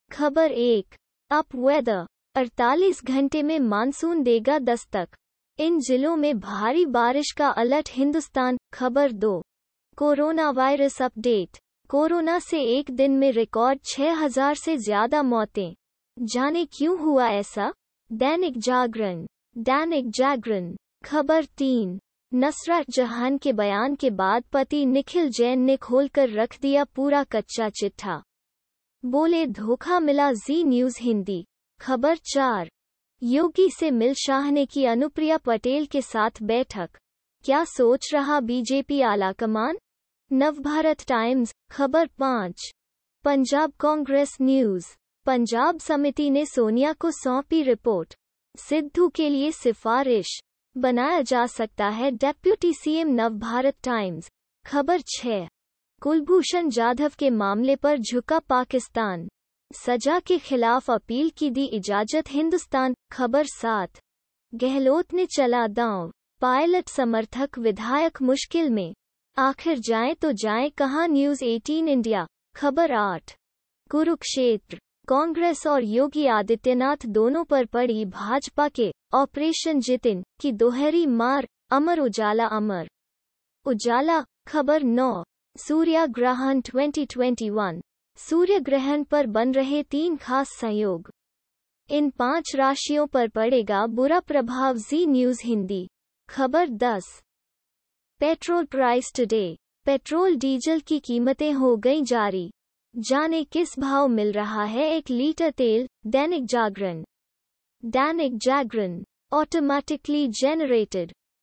Hindi News (भारत)- 11/06/2021,07:30 AM
ख़बर 1:UP Weather : 48 घंटे में मानसून देगा दस्तक, इन जिलों में भारी बारिश का अलर्ट - Hindustan, ख़बर 2:Coronavirus Update: कोरोना से एक दिन में रिकॉर्ड 6 हजार से ज्यादा मौतें; जानें क्यों हुआ ऐसा? - दैनिक जागरण (Dainik Jagran), ख़बर 3:Nusrat Jahan के बयान के बाद पति Nikhil Jain ने खोलकर रख दिया पूरा कच्‍चा-चिट्ठा, बोले-धोखा मिला - Zee News Hindi, ख़बर 4:योगी से मिल शाह ने की अनुप्रिया पटेल के साथ बैठक, क्‍या सोच रहा BJP आलाकमान? - नवभारत टाइम्स, ख़बर 5:Punjab congress news: पंजाब समिति ने सोनिया को सौंपी रिपोर्ट, सिद्धू के लिए सिफारिश... बनाया जा सकता है डेप्‍युटी सीएम - Navbharat Times, ख़बर 6:कुलभूषण जाधव के मामले पर झुका पाकिस्तान, सजा के खिलाफ अपील की दी इजाजत - Hindustan, ख़बर 7:गहलोत ने चला दांव, पायलट समर्थक विधायक मुश्किल में, आखिर जाएं तो जाएं कहां - News18 इंडिया, ख़बर 8:कुरुक्षेत्र: कांग्रेस और योगी आदित्यनाथ दोनों पर पड़ी भाजपा के 'ऑपरेशन जितिन' की दोहरी मार - अमर उजाला - Amar Ujala, ख़बर 9:Surya Grahan 2021: सूर्य ग्रहण पर बन रहे 3 खास संयोग, इन 5 राशियों पर पड़ेगा बुरा प्रभाव - Zee News Hindi, ख़बर 10:Petrol Price today : पेट्रोल-डीजल की कीमतें हो गईं जारी, जानें किस भाव मिल रहा है 1 लीटर तेल - दैनिक जागरण (Dainik Jagran). Automatically Generated... For Advertisement Contact